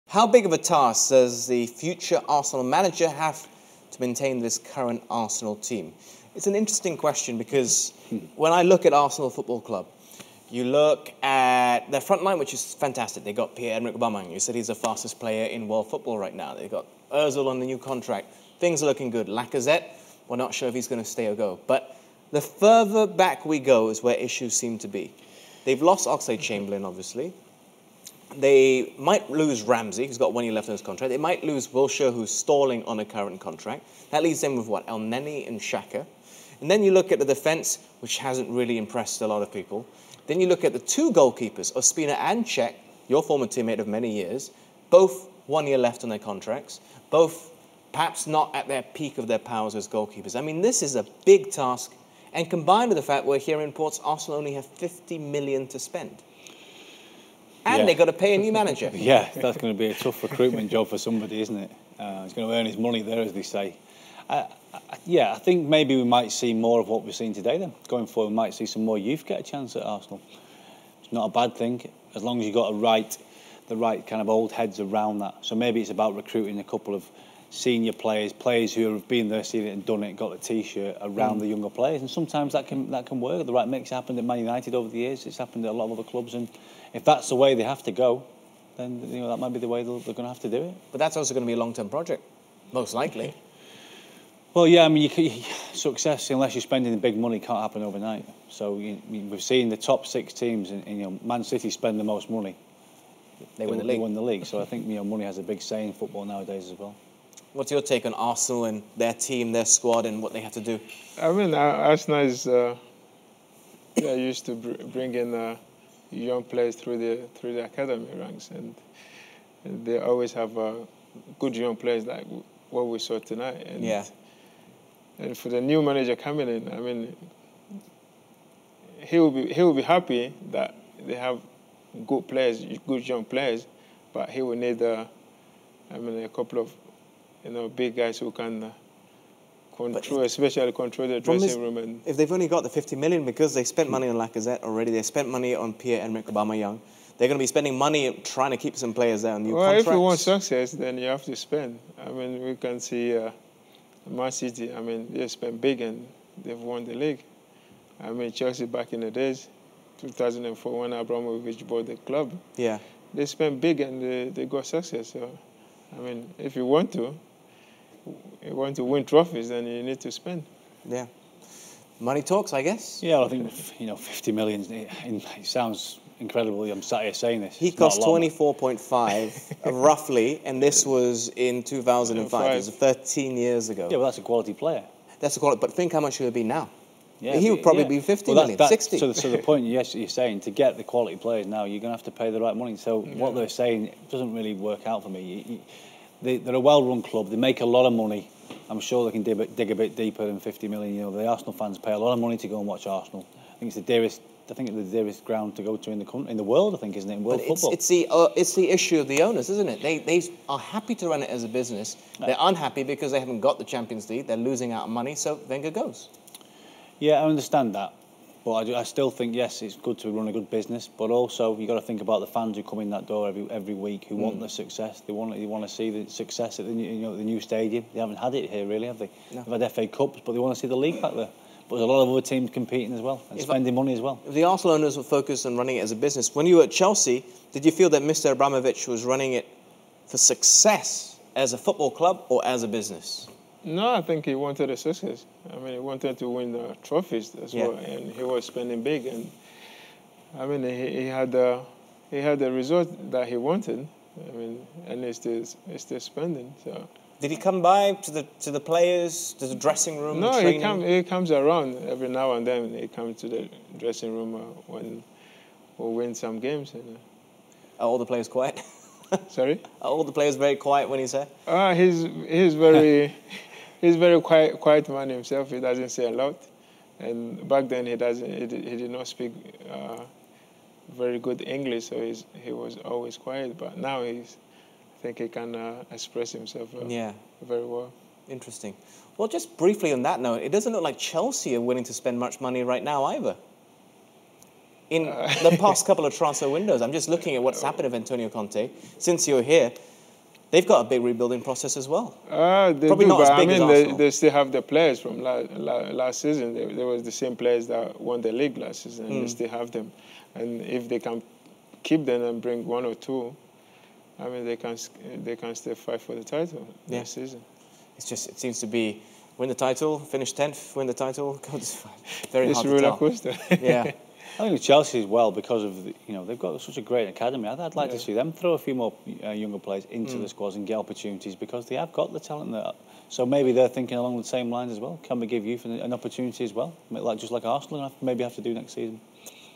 How tough is the rebuilding process? | Man Utd 2-1 Arsenal | Astro SuperSport
What is on the to-do-list for the incoming manager at Arsenal to speed up the rebuilding process at the club? Michael Essien and Dominic Matteo ponder on the post-Wenger era.

Essien was in Malaysia for Astro SuperSport's live EPL coverage.